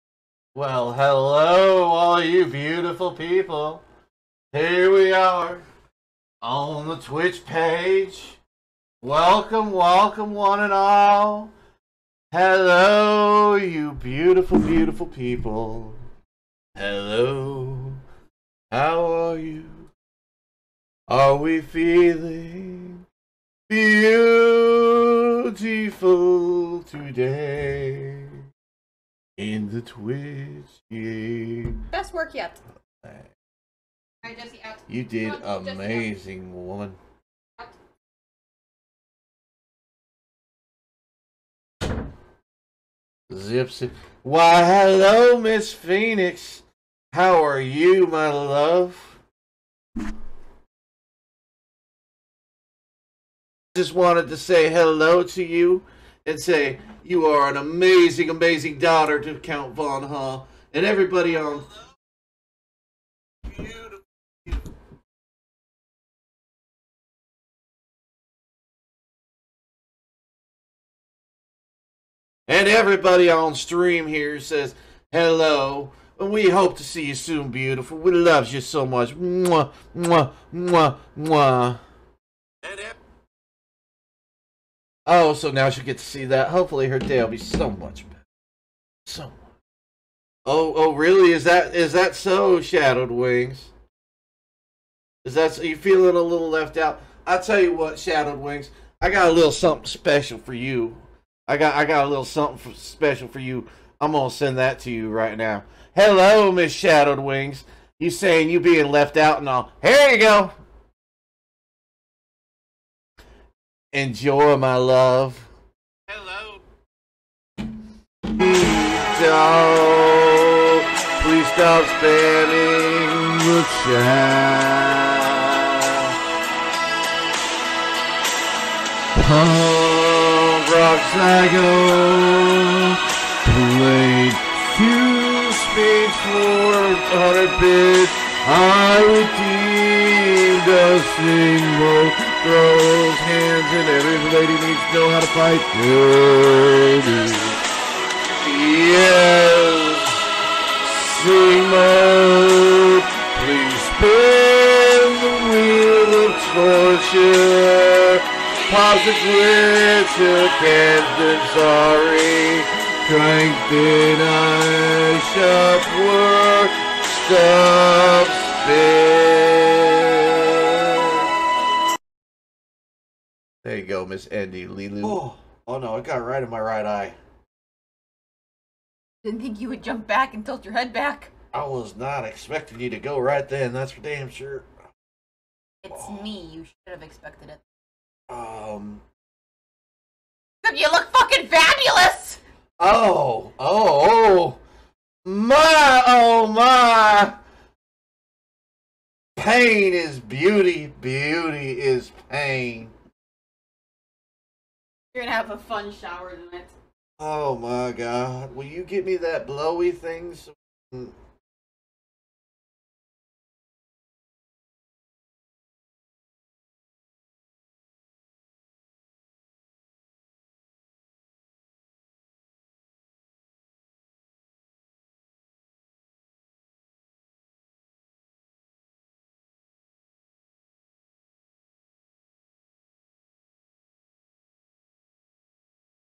Streaming / live show / twitch/ drag queen / OC / Cruella Von Huh / countskids / discord / tiktok /
We gave the entire Count Von Huh live show to Cruella Von Huh for the night. Here are some of the highlights